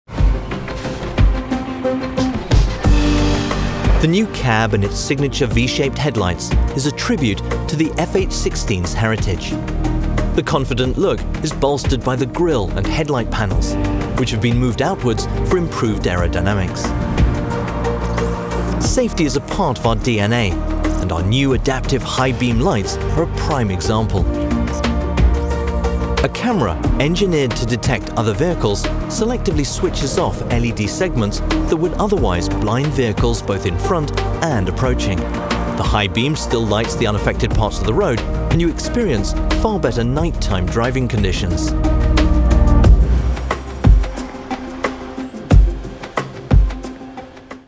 Volvo Trucks – The new Volvo FH16 - Headlight heritage
The new cab and its signature V-shaped headlights is a tribute to the new Volvo FH16’s heritage.

The confident look is bolstered by the grill and headlight panels, which have been moved outwards for improved aerodynamics. Our new adaptive high-beam lights are connected to a camera and radar, engineered to detect other vehicles.

LED segments are selectively switched off to avoid blinding drivers of vehicles both in front and approaching. The high beam still lights the unaffected parts of the road and you experience far better night-time driving conditions.